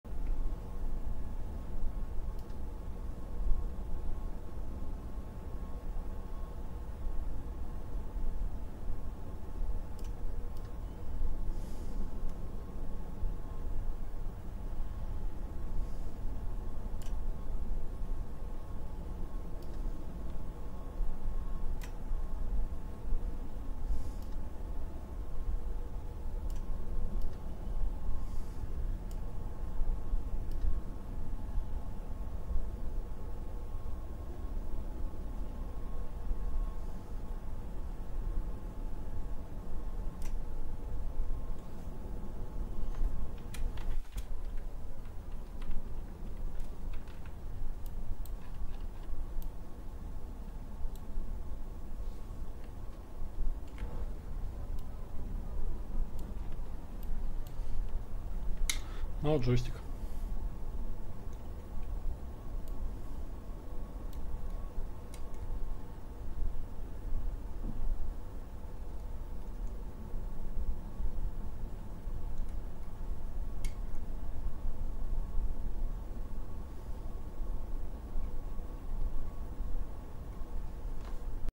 Writing logic for stab trim on B747-200
Just started making logic for elevator trim lever and default commands. So far so good :)